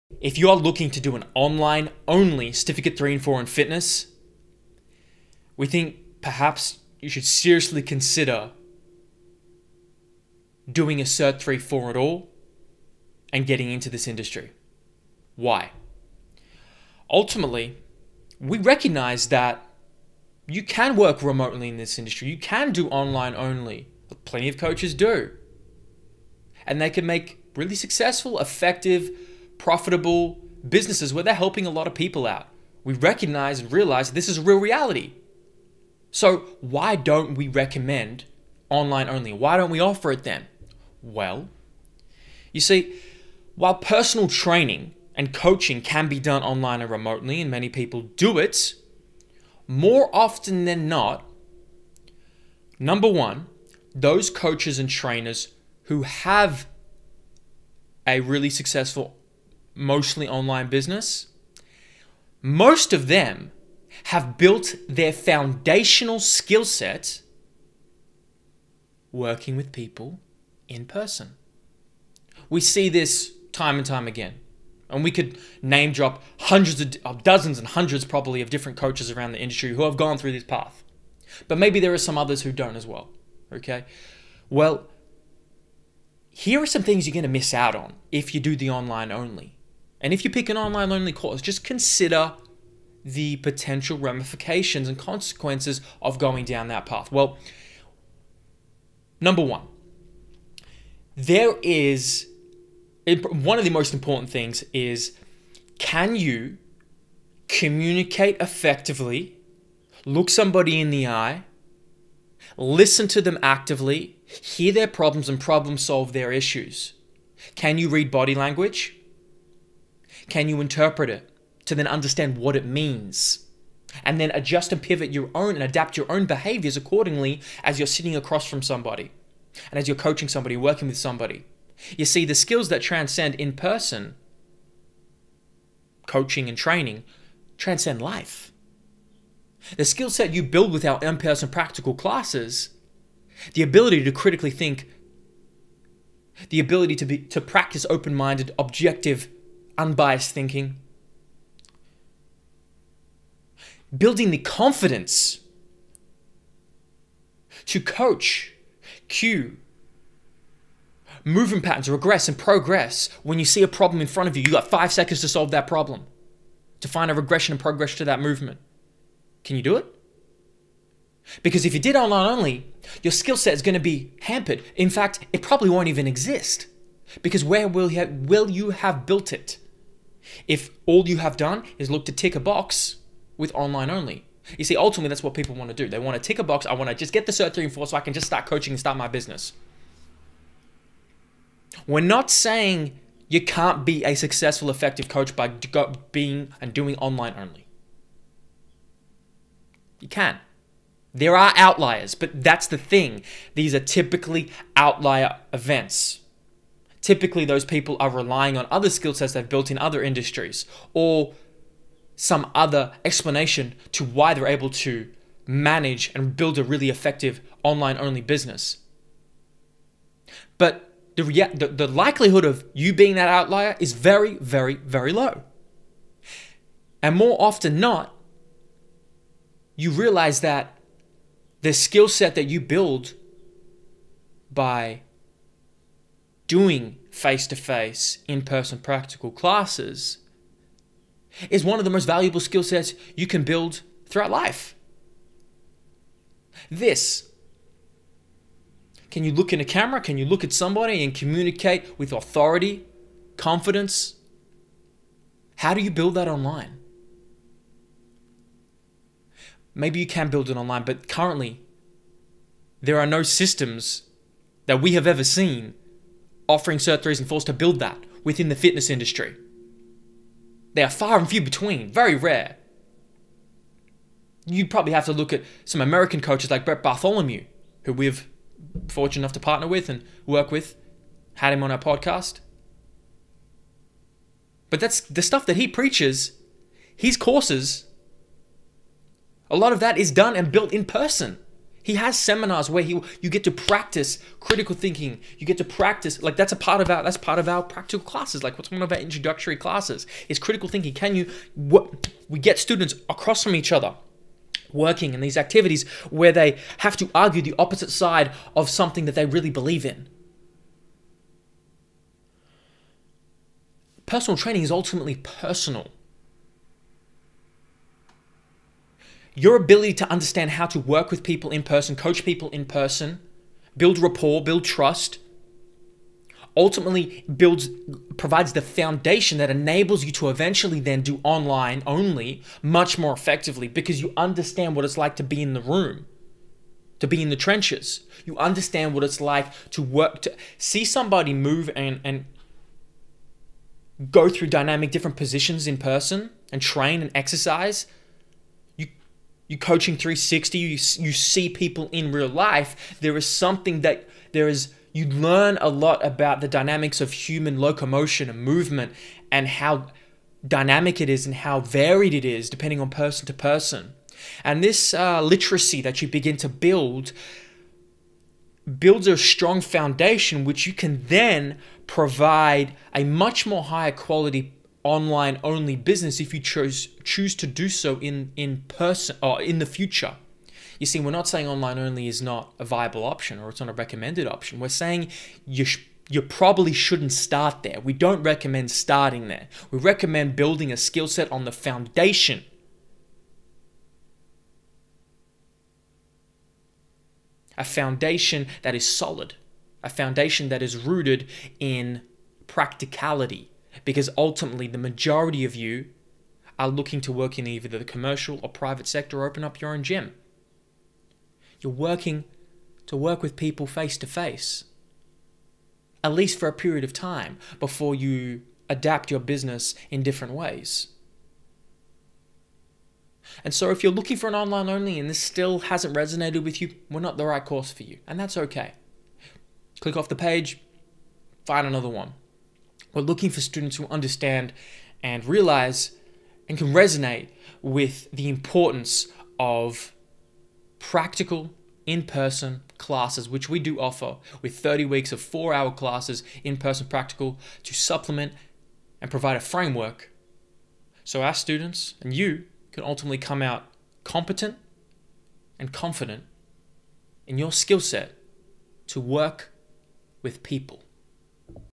Why You Should NOT Do Your Certficate III/IV In Fitness Online!
Why an online-only Certificate III/IV in Fitness could be the biggest mistake of your career. To inquire about our Certificate III/IV in Fitnenss and find out more head

Orphic Education is one of the only RTO’s that do NOT deliver an online-only course. Why?

The foundation of a personal trainer’s skillset relies on the practical application of solving problems, communicating and coaching IN-PERSON.

We also notice people best learn and retain information when challenged in a dynamic real-life setting.

This is why we deliver 30 weeks of weekly practical classes on top of our online learning system.

With that being said, most Certificate III/IV’s in Fitness in Australia has a compulsory online component that entails you completing a variety of modules.

Inside these are modules are the foundations of theoretical knowledge you need to know as a PT.

Within these are assessments such as multiple-choice and short answer questions and a variety of case study type assessments, some of which you complete in person under the guidance of a qualified personal trainer.

This will give you the opportunity to test and practice the skills of program writing, exercise prescription and implementation.

Many of these assessments are completed within your practical classes in combination with any other gym or trainer that you seek out.